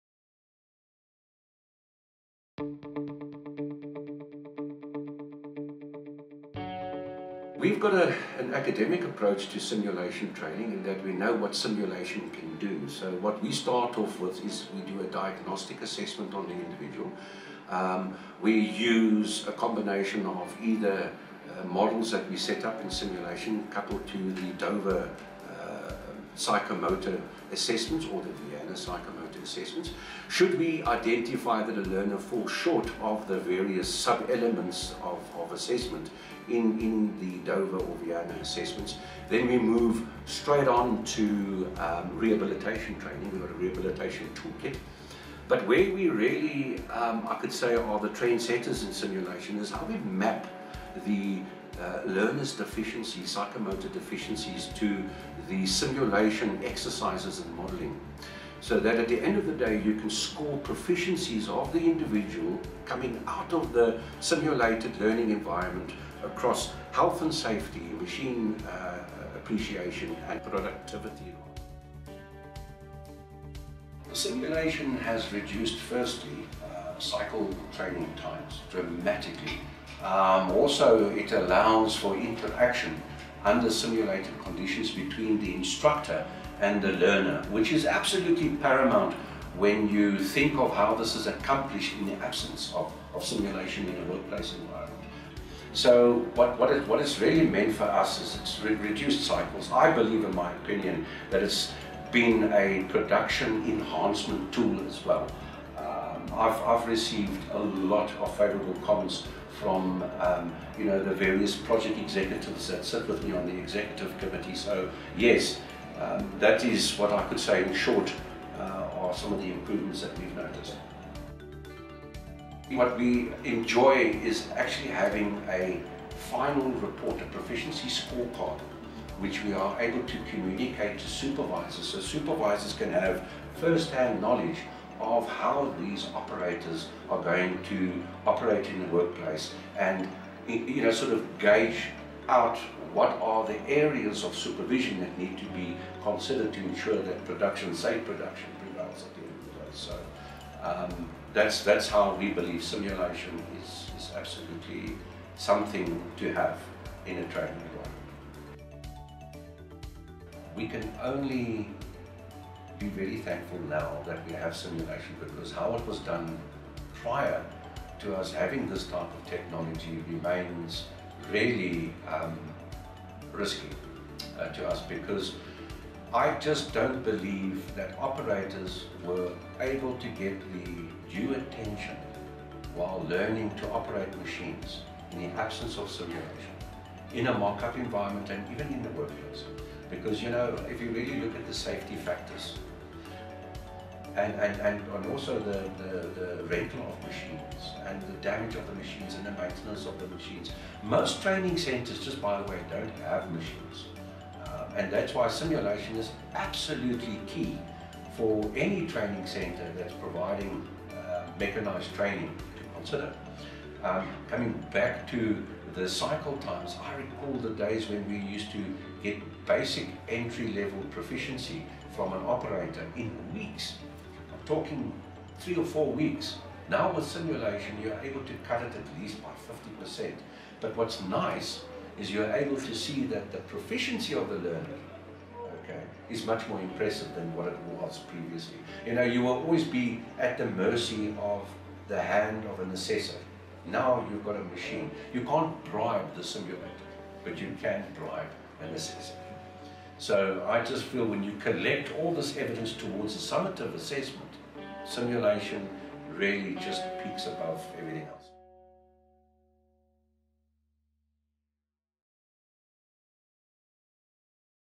Murray & Roberts Training Academy Realising the Benefits of CYBERMINE Simulators
Murray & Roberts is internationally recognised as one of the world's leading mining contractors with operations in Africa, Australia and the America's. By utilising CYBERMINE simulators the Murray & Roberts Training Academy ensure their workforce is both safe and proficient.

Find out how CYBERMINE mining simulators can improve the safety and productivity of your workforce